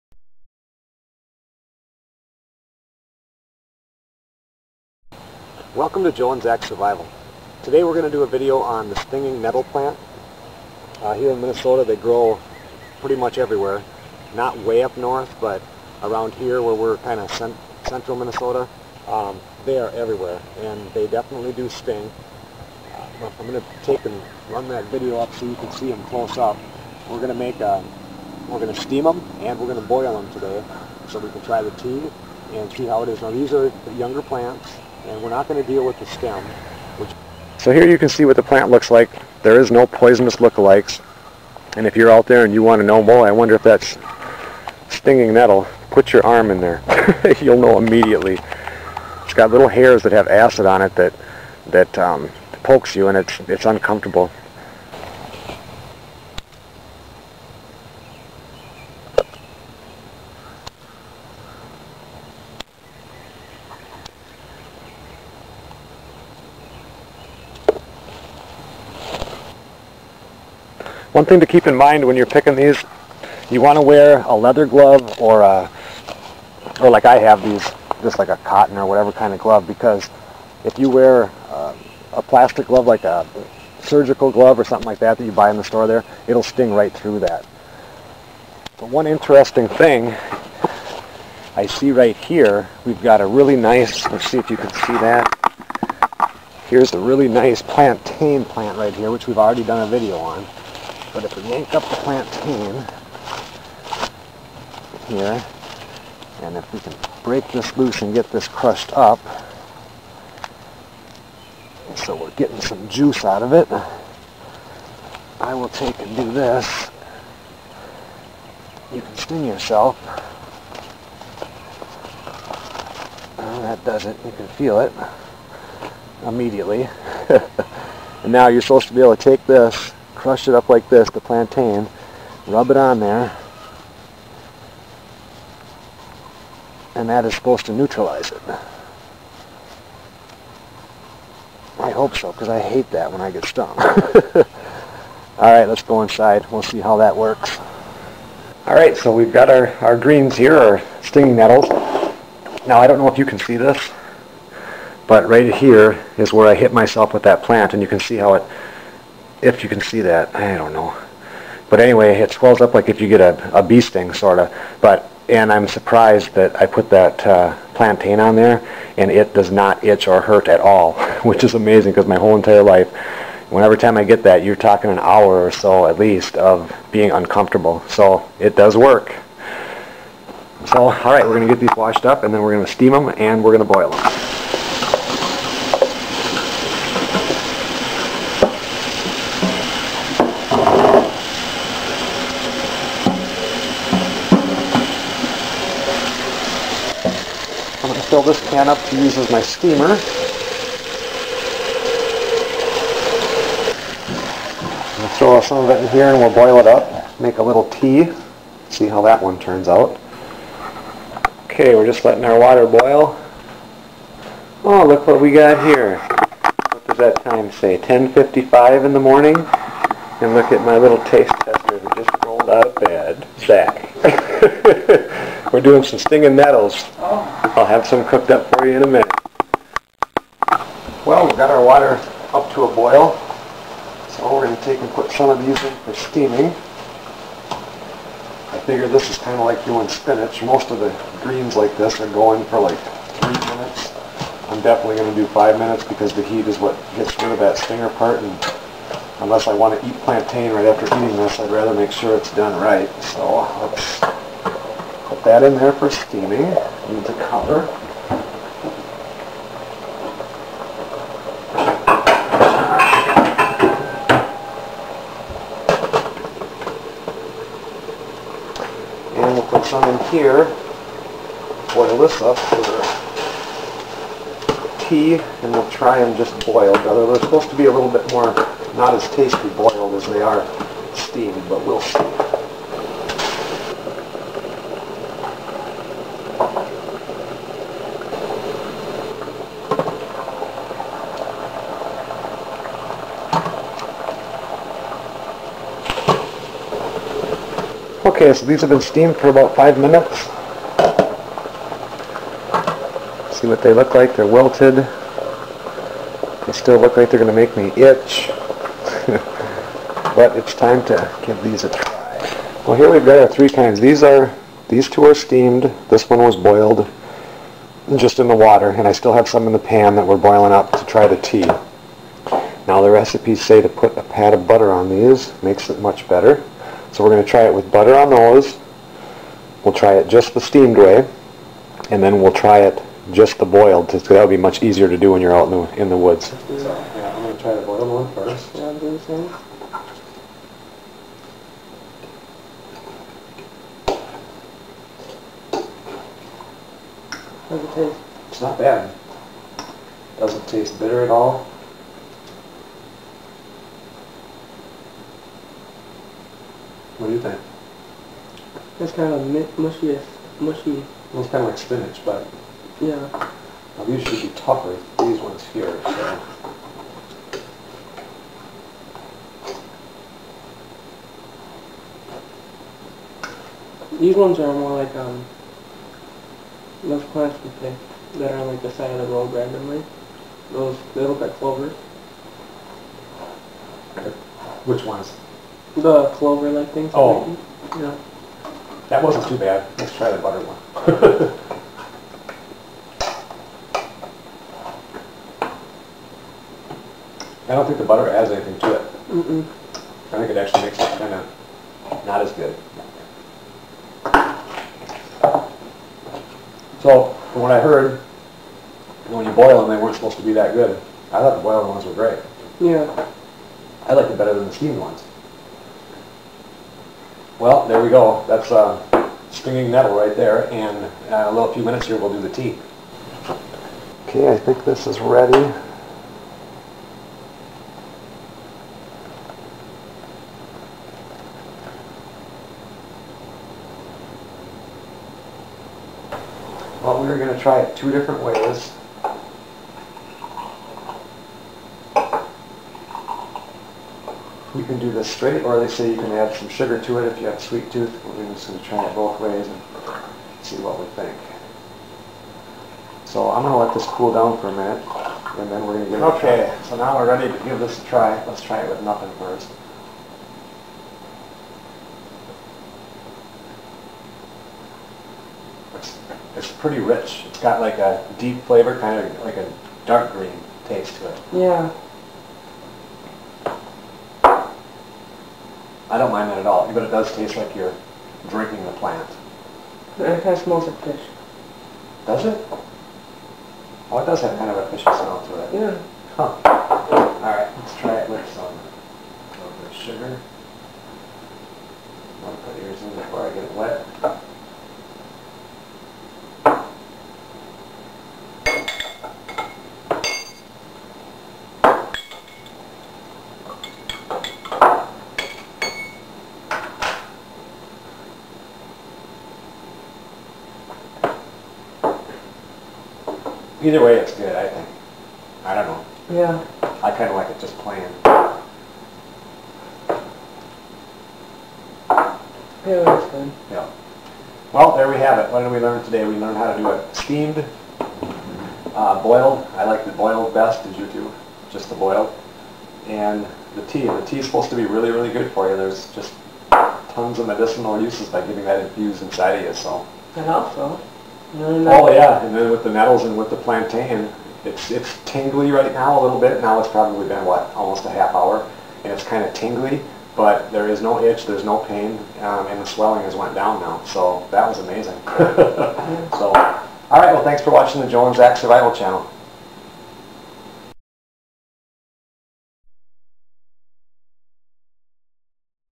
Wild Edibles: Stinging Nettle, boiled, steamed and made into tea
This video features Stinging Nettle as a wild edible, from identification to cooking by boiling and steaming and then making a sweetened and unsweetened tea, we try it all. I also " sting " myself and try using Plantain to counter the effects. Enjoy the video and Thank You.